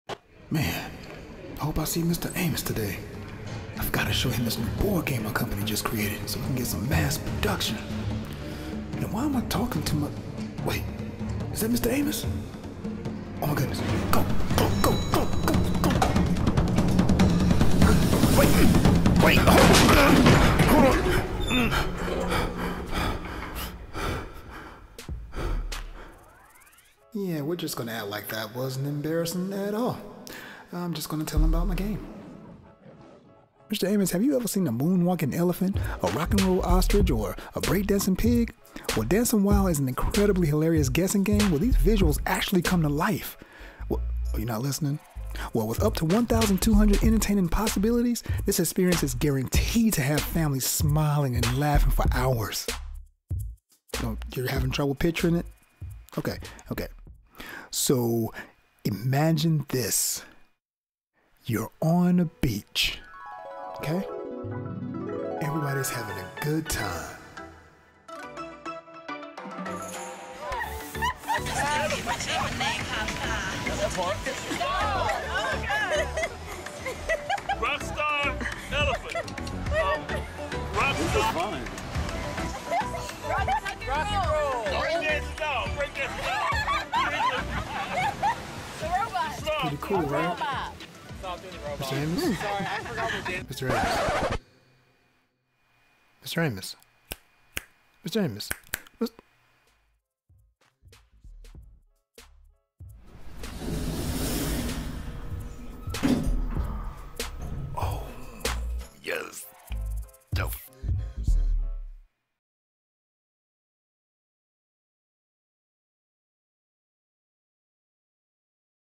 Ingredients For Success (funny Dancin' Wild "Elevator Pitch")
Have you ever seen a moonwalking elephant? A rock-n-roll ostrich? What about a breakdancing pig? Well, Dancin' Wild is an incredibly hilarious guessing game where these visuals come to life! With up to 1,200 entertaining possibilities, this experience is guaranteed to have families smiling and laughing for hours; providing priceless memories!